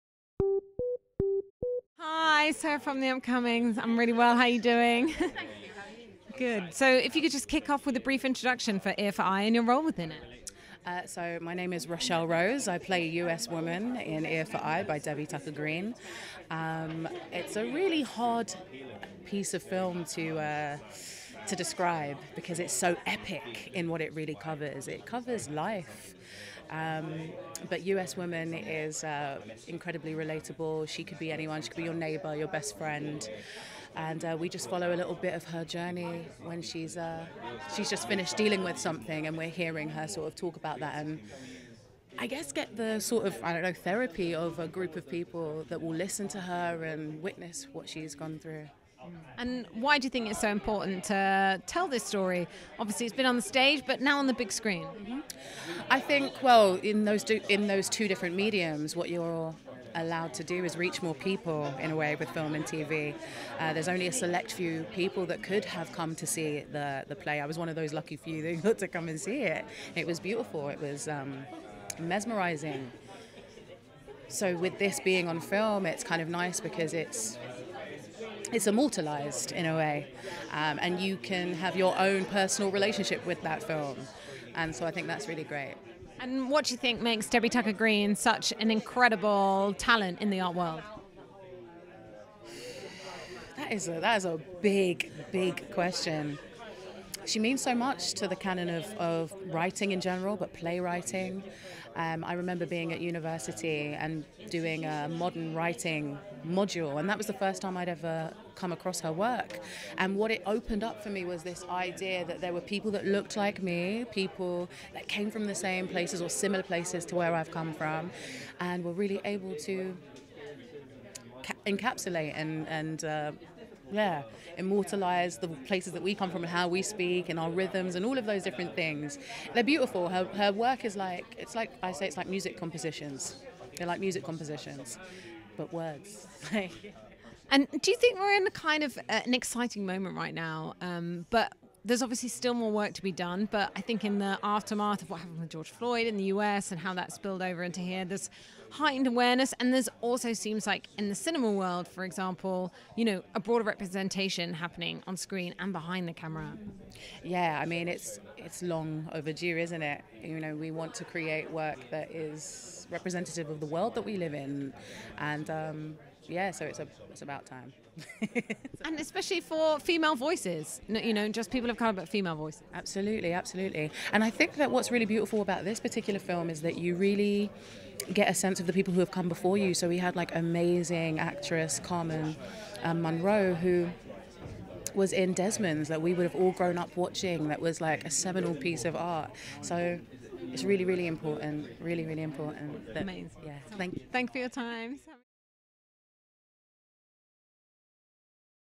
Kelechi Okafor on Ear for Eye at London Film Festival 2021 premiere interview
Kelechi Okafor on Ear for Eye at BFI London Film Festival 2021 premiere interview